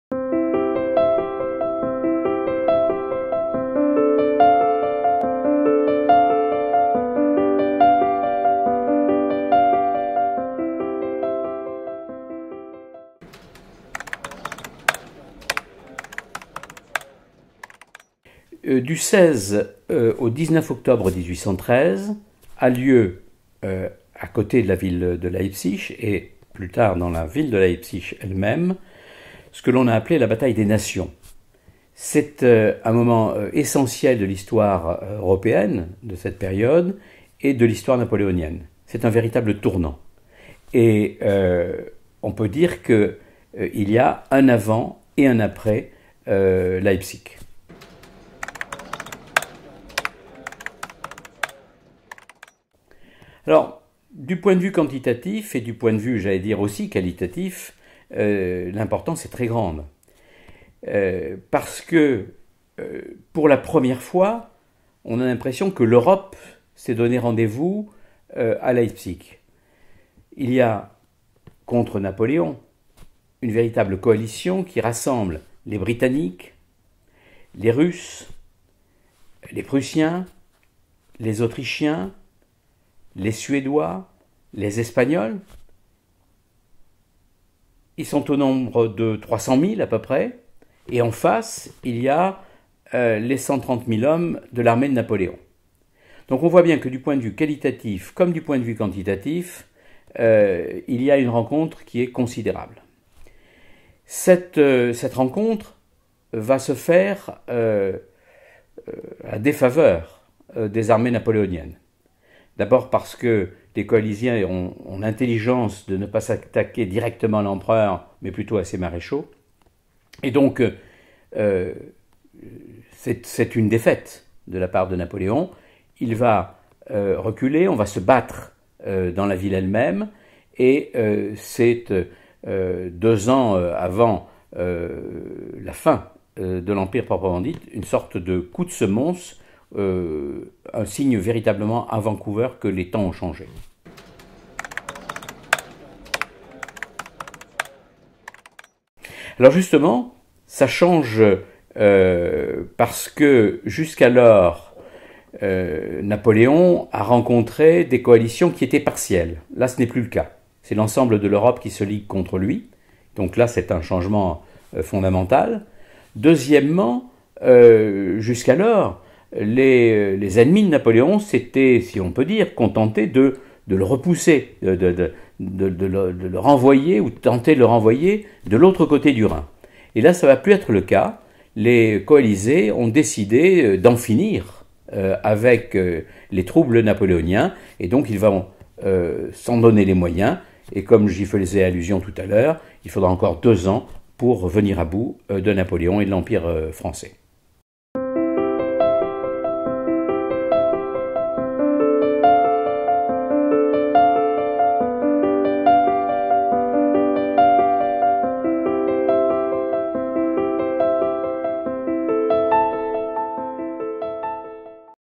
Leipzig -- 16-19 octobre 1813, Bruyère-Ostells Walter
- Qu'est-ce que cette bataille ?
- Quelle fut son importance ?
- Que change t-elle ?